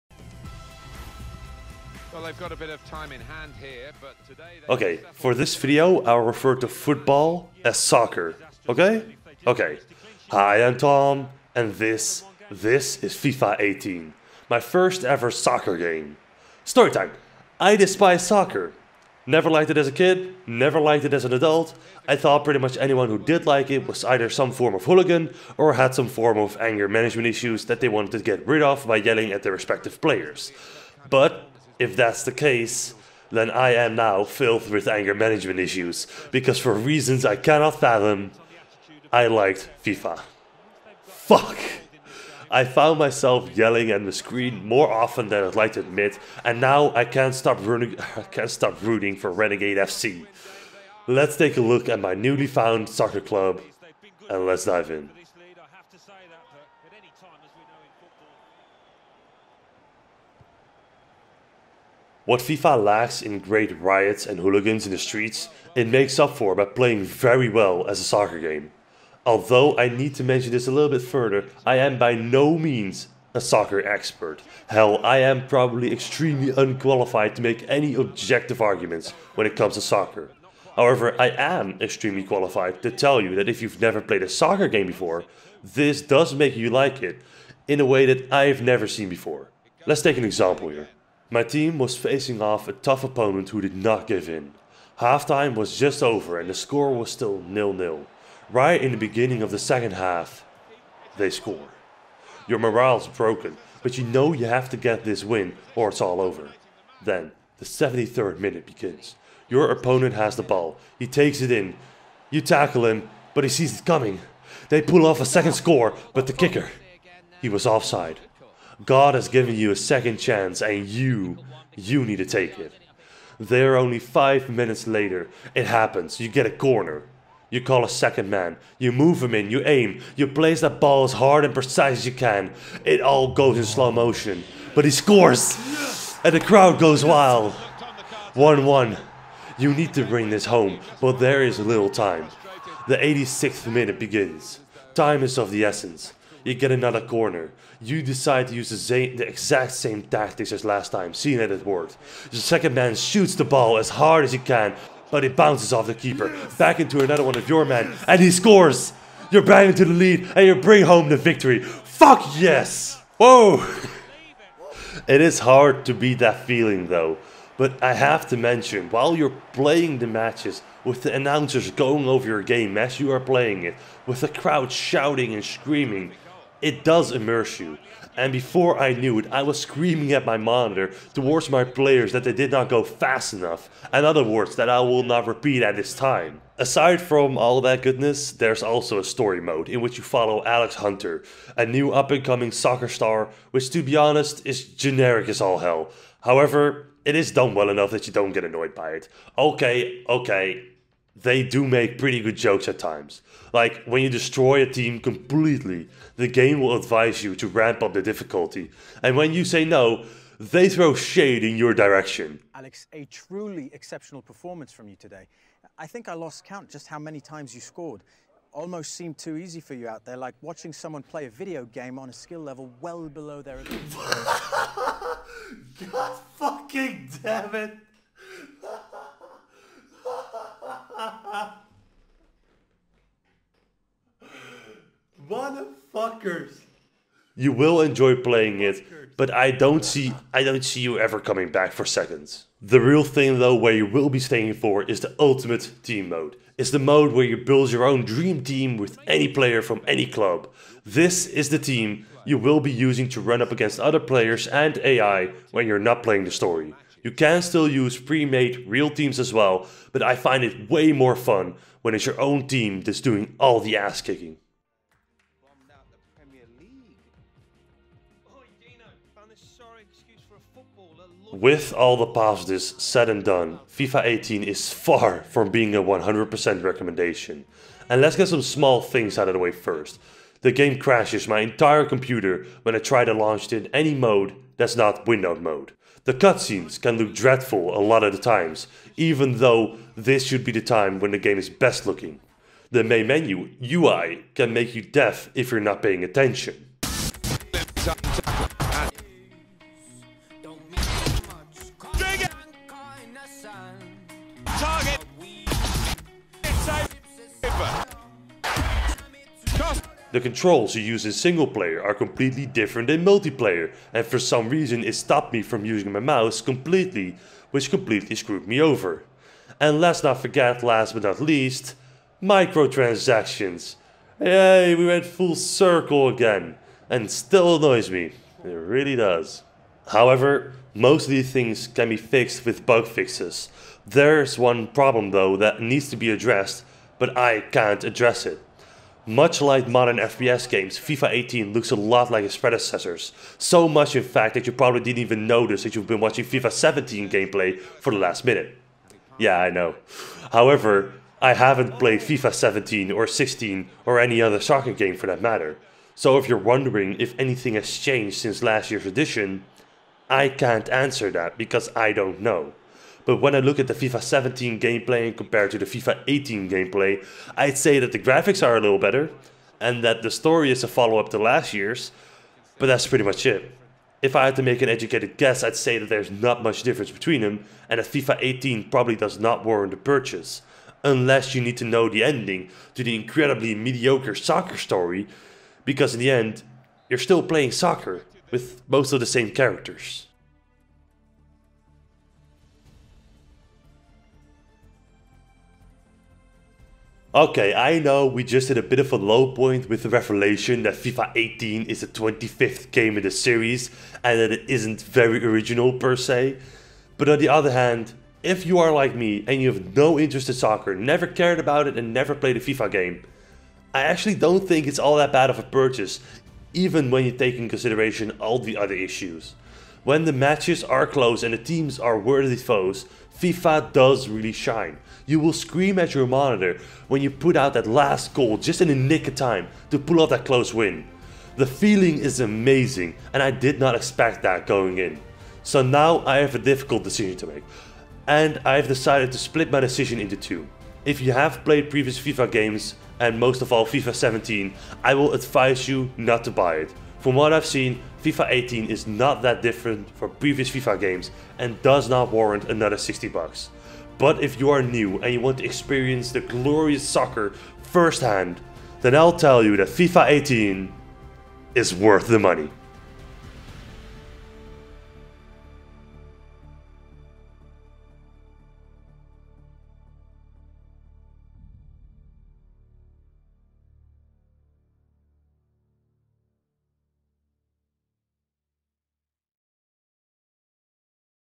(REVIEW) Is FIFA18 Worth The Money?
I never played any sports, sports games, nor have I dreamed of becoming an athlete... So it is now, that i have played a sports game for the first time. And it was better than I expected.